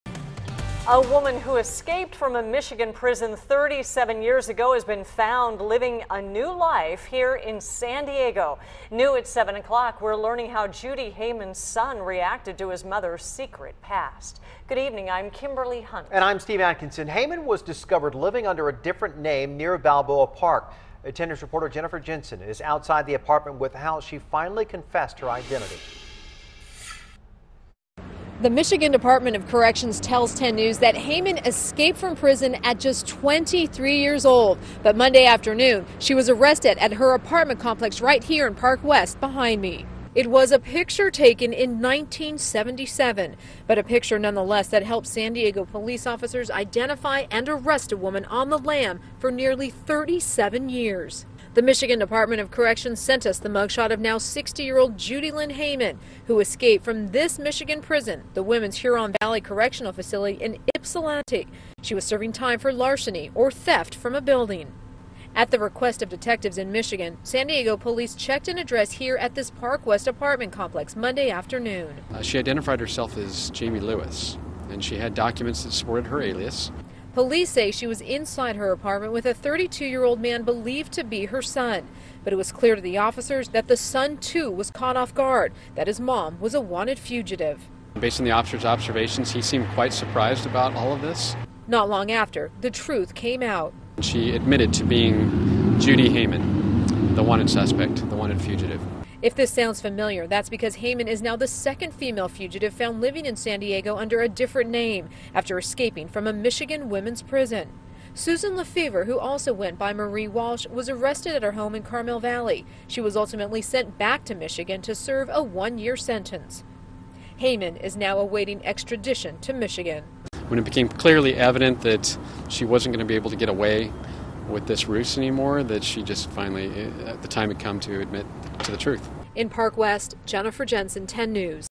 Judy Lynn Hayman arrested in San Diego after allegedly escaping Michigan prison in 1977
A woman who allegedly escaped from a Midwestern prison 37 years ago was captured in the Balboa Park-area neighborhood where she was living, authorities reported Tuesday.